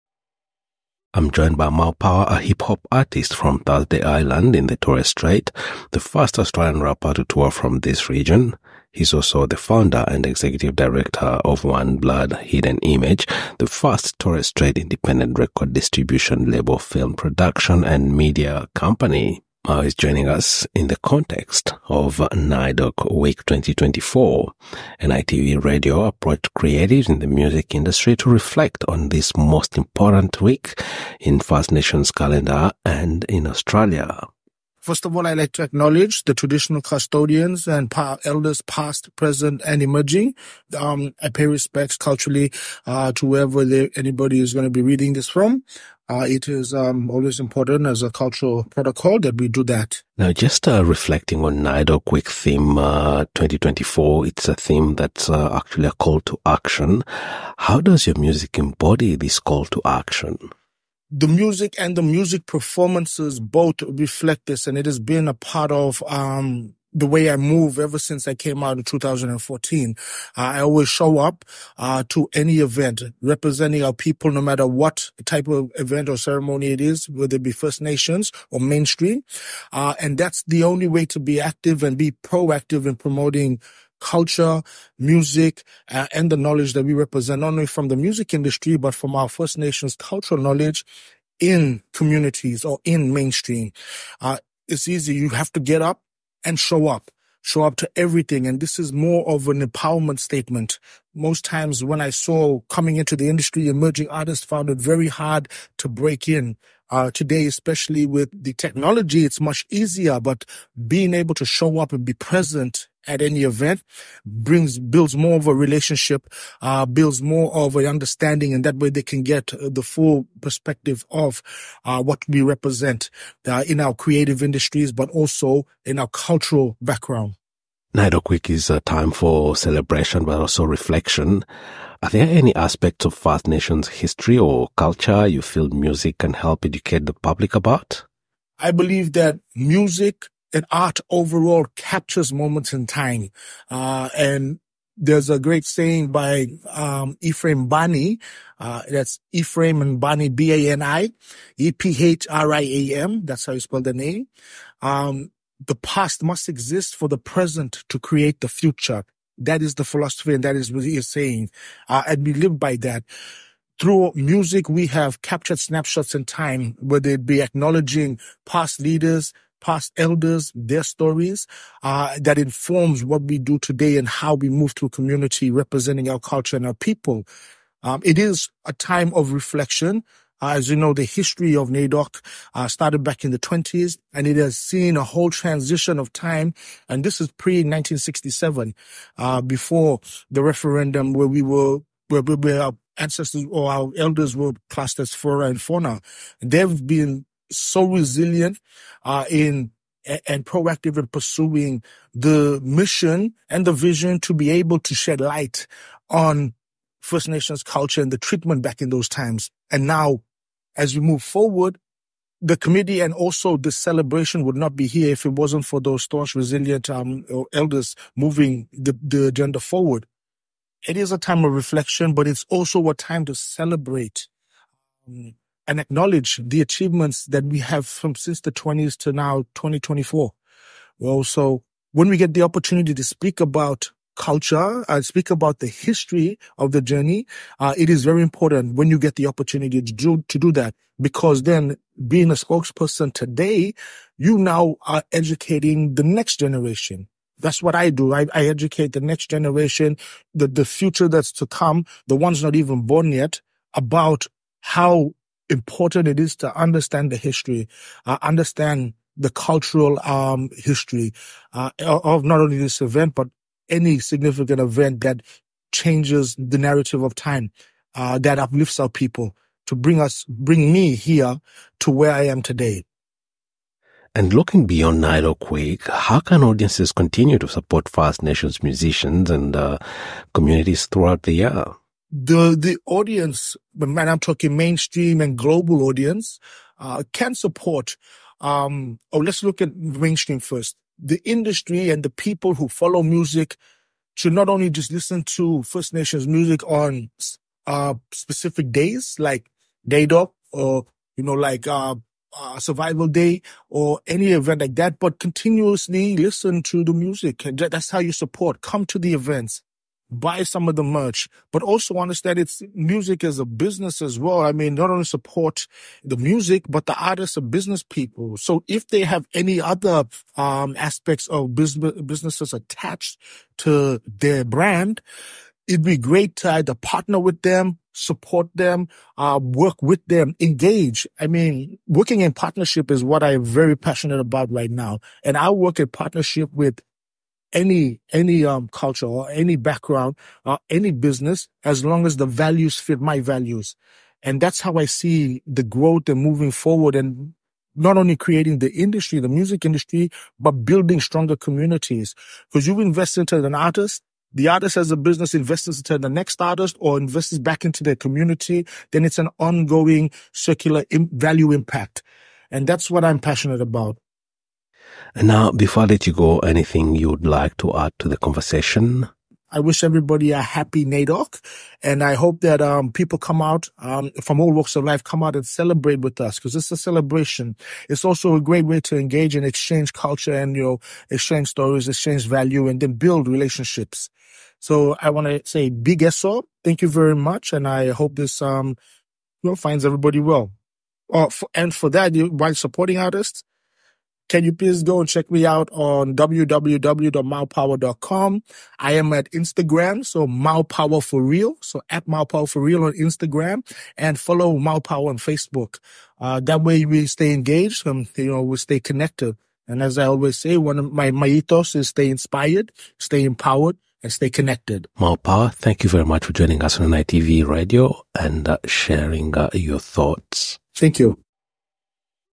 Mau Power: on NAIDOC Week get up, show up and showcase culture | SBS NITV Radio
“When we get the opportunity to speak about culture, speak about the history of the journey it is very important to take that opportunity because, if you are the spokesperson today you are educating the next generation.” – Mau Power